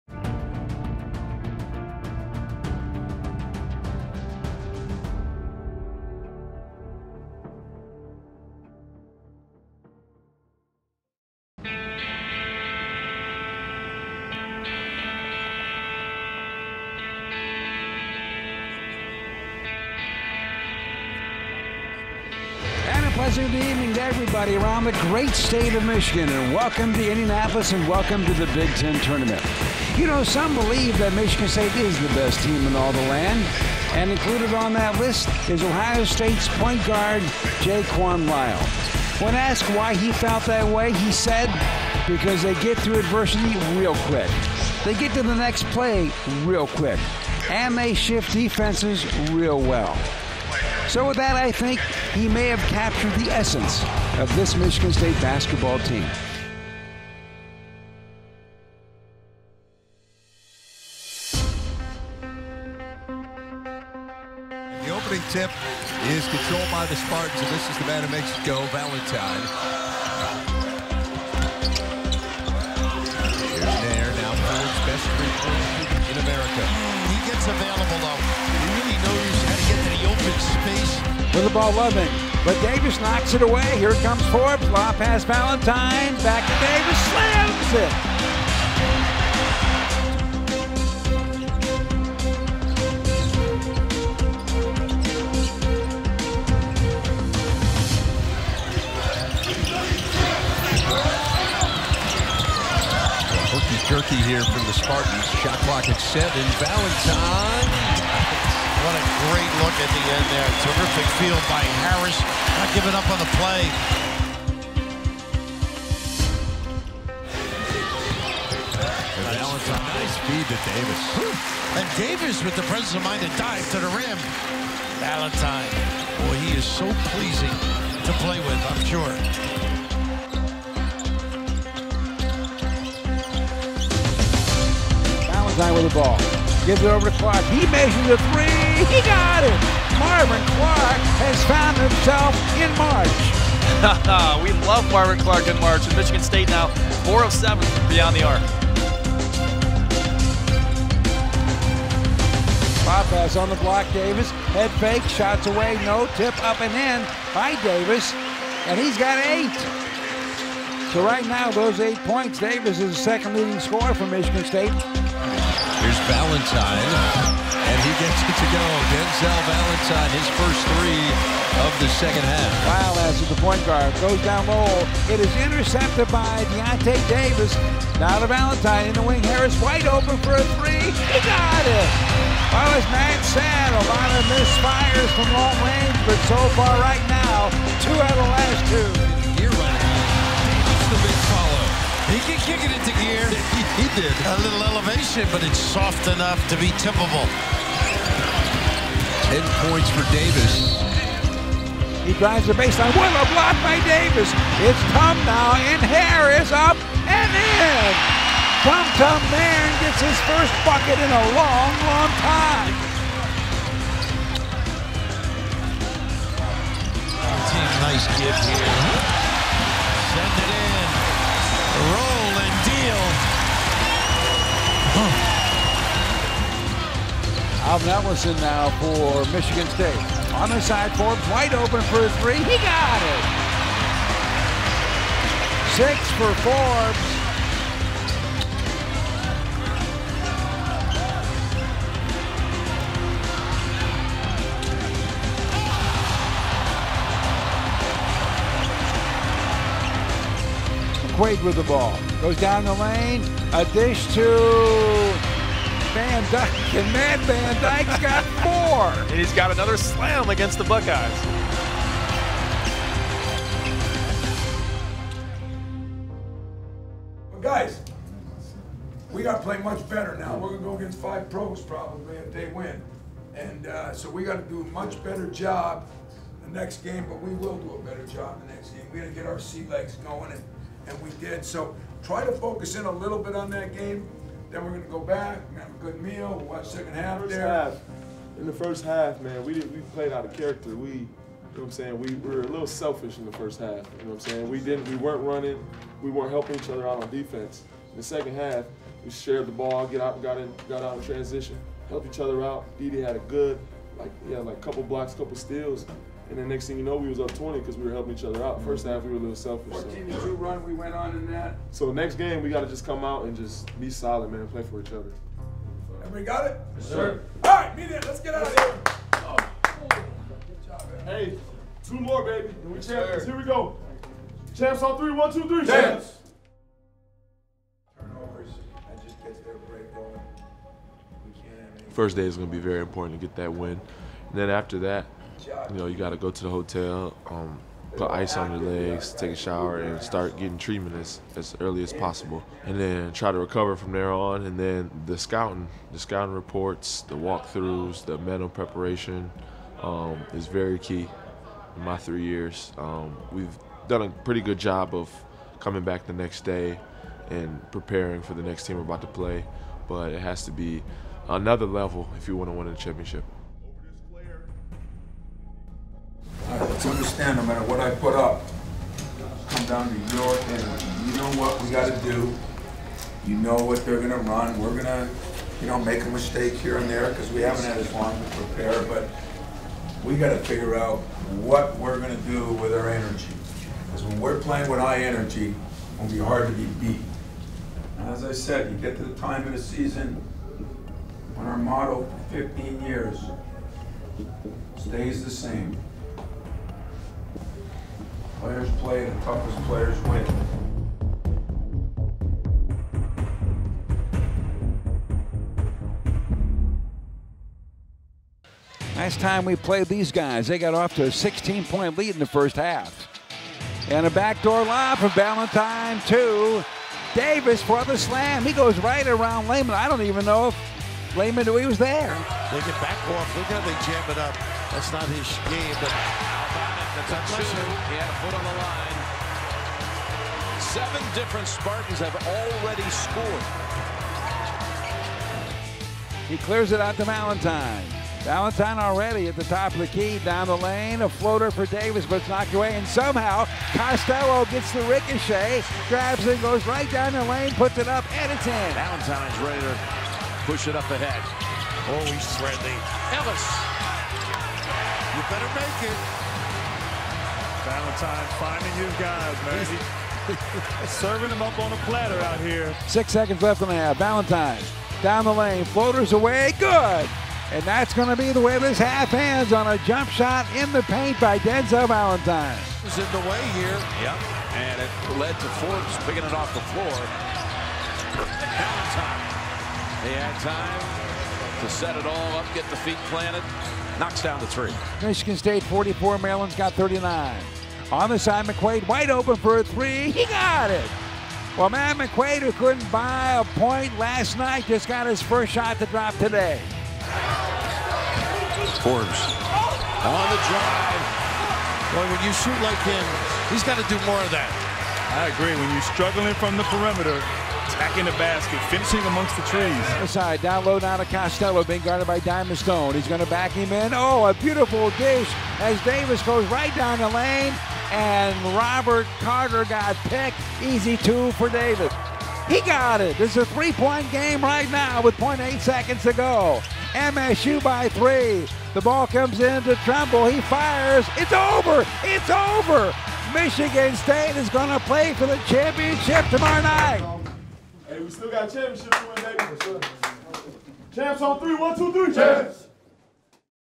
Spartan Basketball All-Access: "Tough Players Win"
In this special web episode go behind the scenes with the team as they face off in the Big Ten Tournament vs Ohio Stat and Maryland.